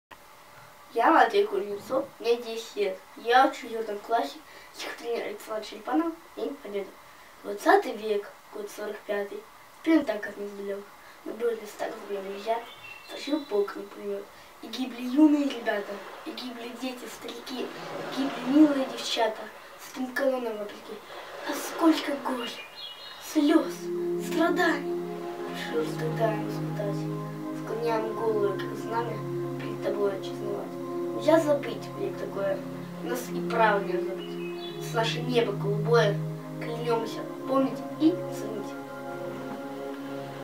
Матвей Кузнецов Стих День Победы
Конкурс «История России в стихах». Российское историческое общество.